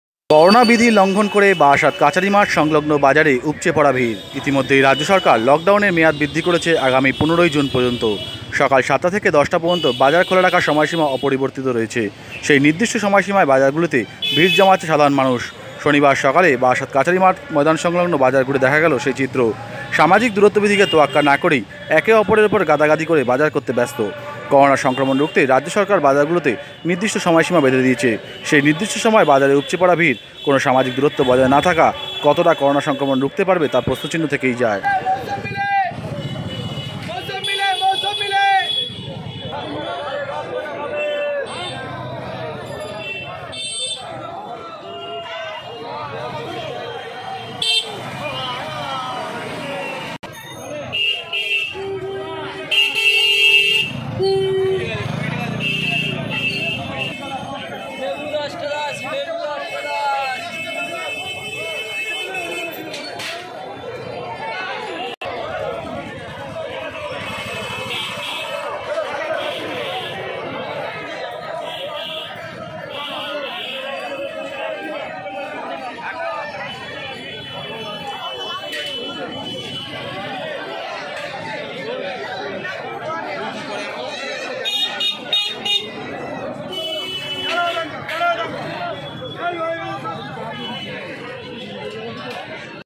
করোনা বিধি লংঘন করে বারাসাত কাছারি ময়দান সংলগ্ন বাজারে উপচে পড়া ভিড়lockdown Barasat covid#
করোনা বিধি লংঘন করে বারাসাত কাছারি ময়দান সংলগ্ন বাজারে উপচে পড়া ভিড়